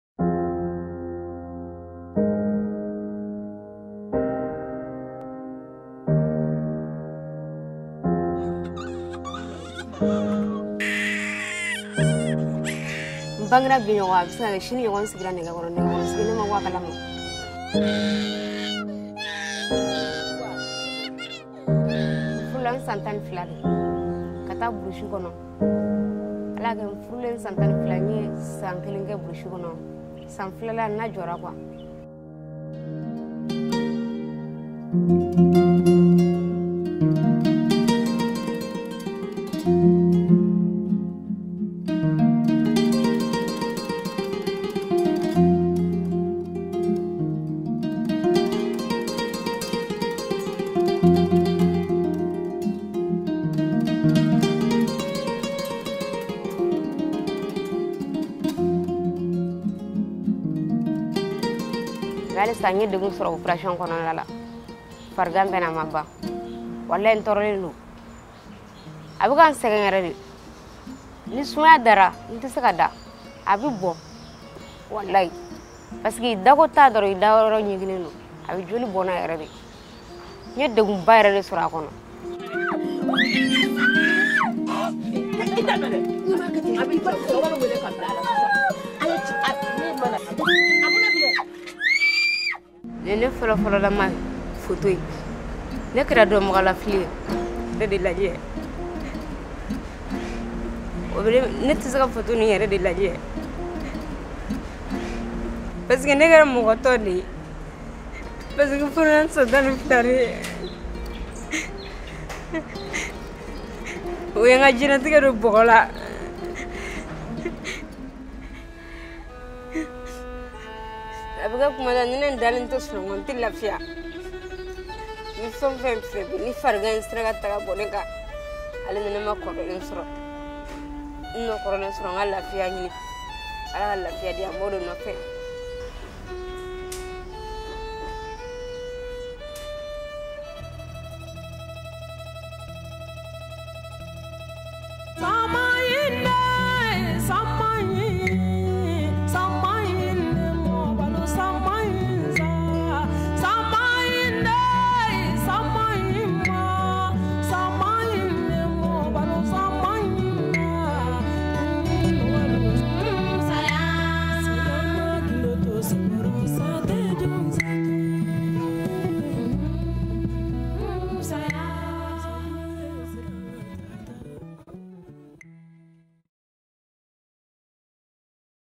Mali film for GMC's opening FGM conference Sept 2019 - ENG version
A moving testimonial from a victim of Female Genital Mutilation shot by the Global Media Campaign to End FGM and shown in Mali in September 2019 at their anti-FGM conference